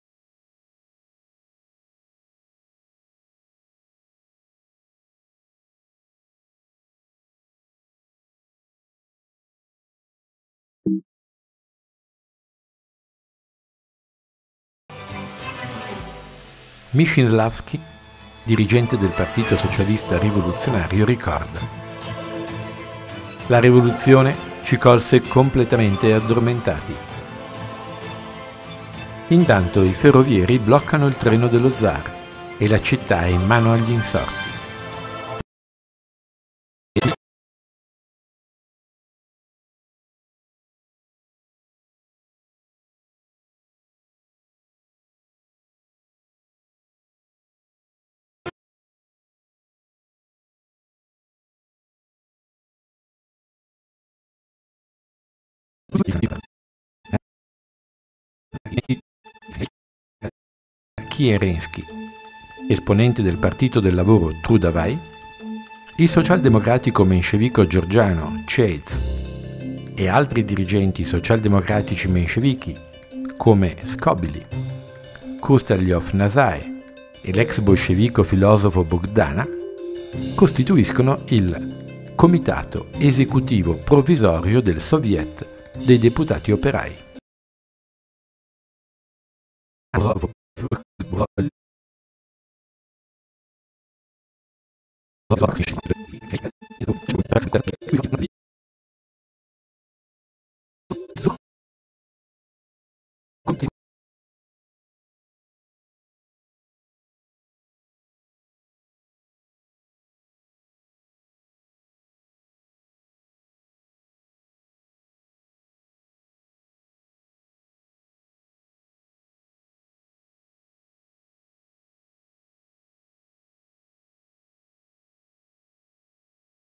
Split Screen Esempio di montaggio su più tracce
Come creare uno split screen con DaVinci Resolve leggi il blog, scarica il Magazine, ottieni altri contenuti.